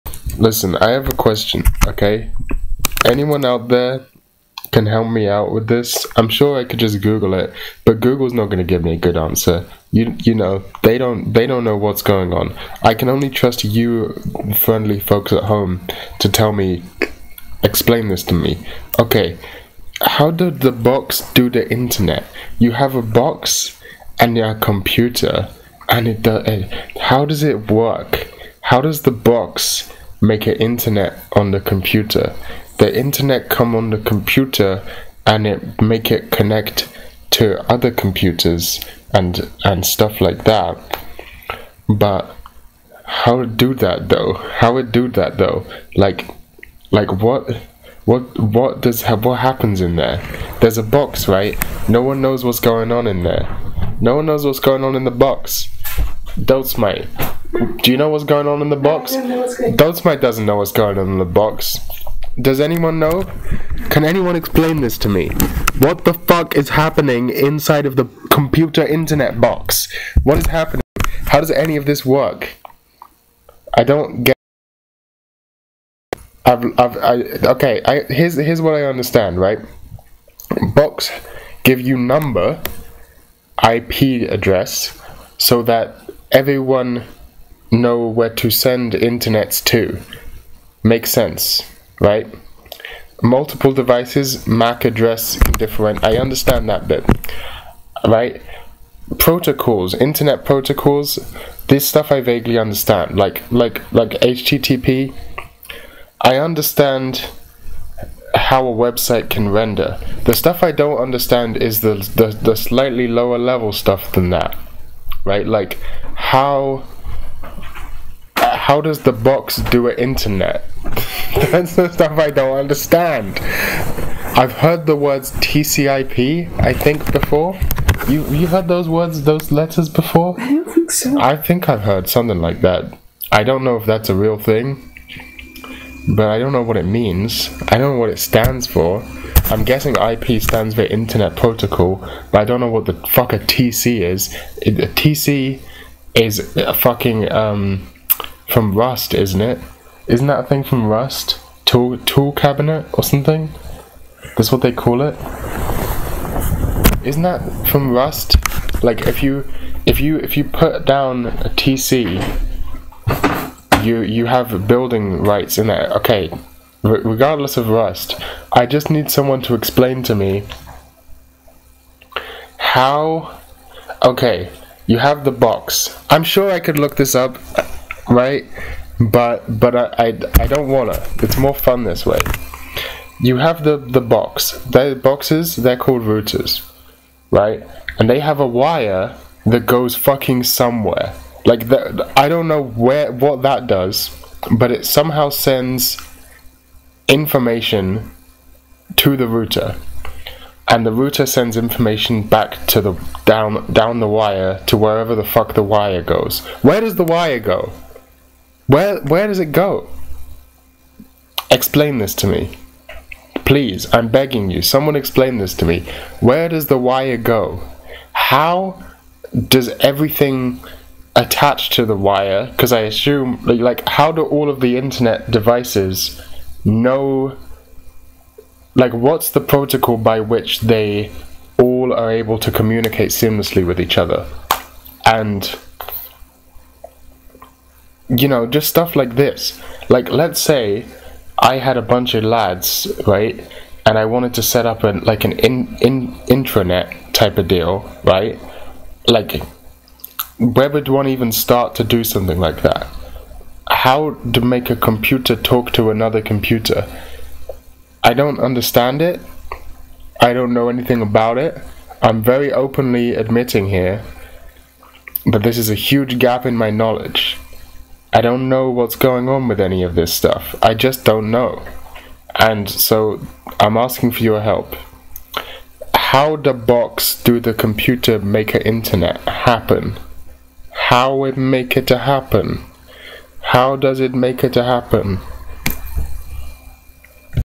someone explain to me how internet works?? please
EXPLAIN INTERNET TO ME OR I'LL FUCKING KILL YOU! DON'T DUMB IT DOWN INTO SOME VAGUE SHIT! EXPLAIN INTERNET TO ME RIGHT NOW OR I'LL LITERALLY FUCKING KILL YOU! WHAT THE FUCK IS A ROUTER? WHAT THE FUCK ARE PORTS? DON'T DUMB IT DOWN OR I'LL FUCKING KILL YOU